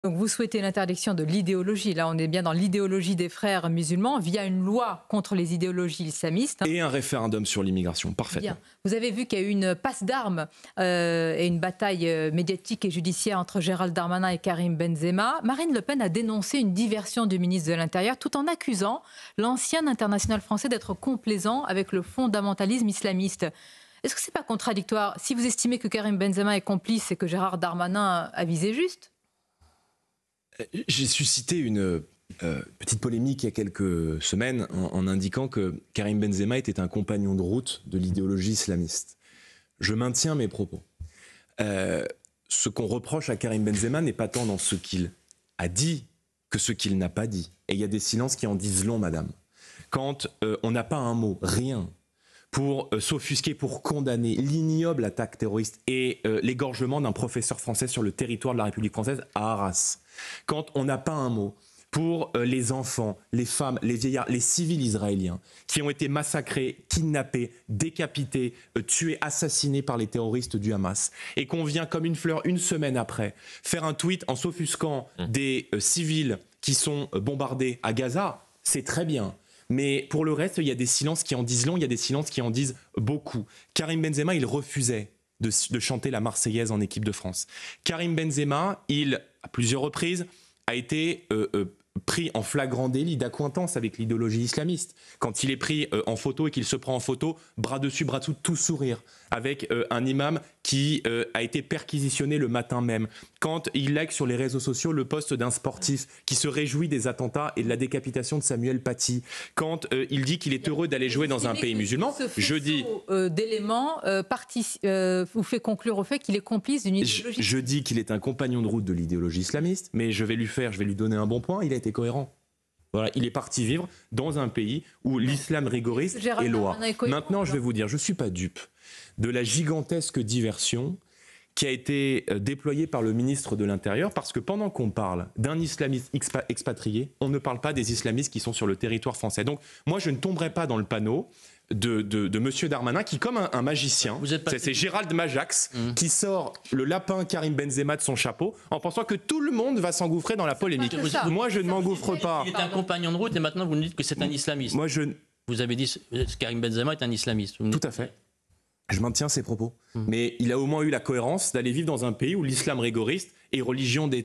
Jordan Bardella : "Karim Benzema est un compagnon de route de l'idéologie islamiste"
«Karim Benzema est un compagnon de route de l'idéologie islamiste», réaffirme Jordan Bardella